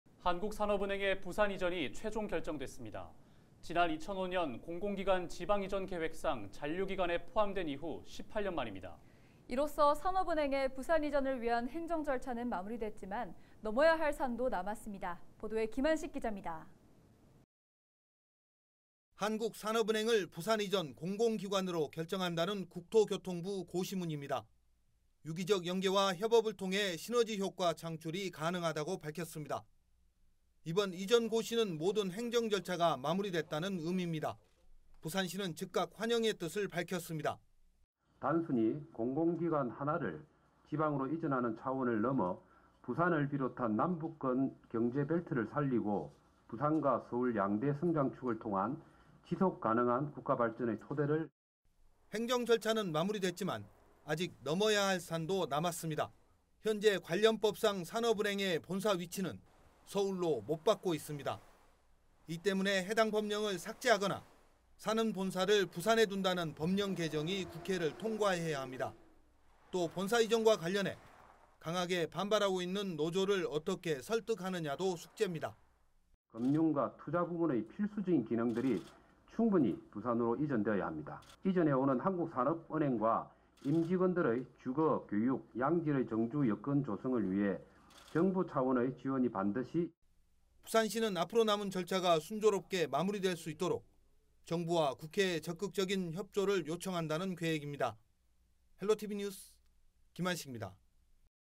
한국산업은행 '부산행' 첫 발 뗐다!
산업은행이 이전 공공기관으로 지정 고시되면서 부산 이전에 속도가 붙게 됐습니다.
산업은행 노조의 반대와 본점 소재지를 서울로 규정한 산업은행법 국회 개정은 숙제로 남았습니다.